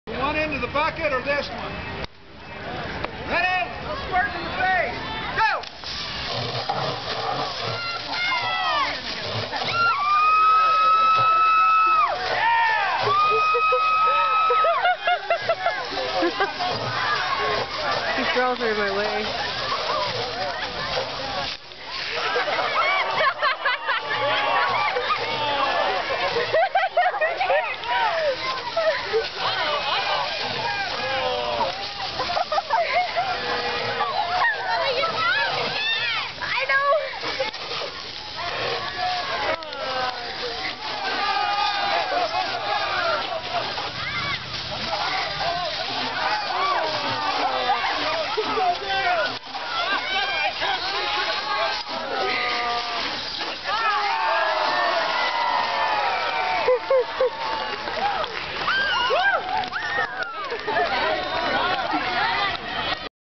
Pwned by fire hose
Murray Utah Fire fighters made the forth of july the best this year.